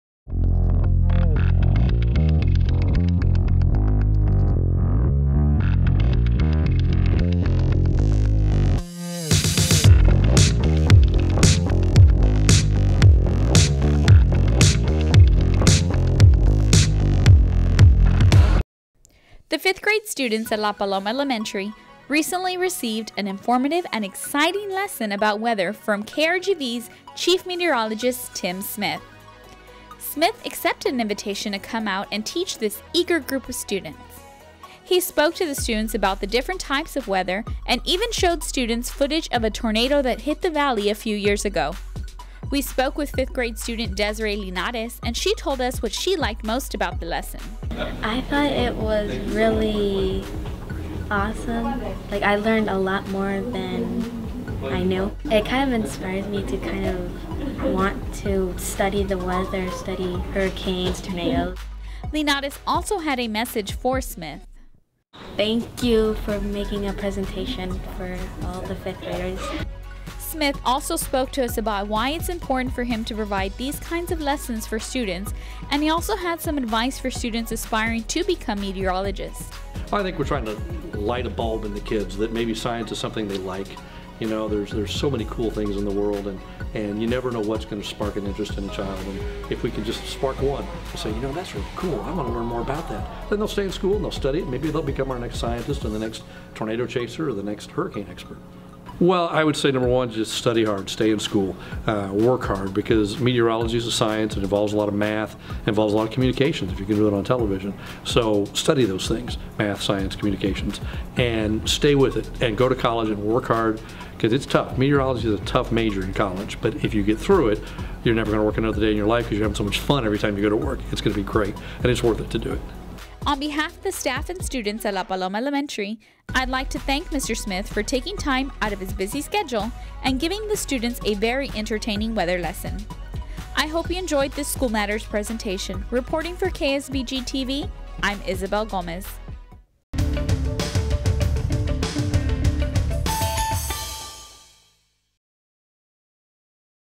KRGV's Tim Smith visits La Paloma Elementary
The 5th grade students at La Paloma Elementary recently received an informative and exciting lesson about weather from KRGV'S Chief Meteorologist Tim Smith.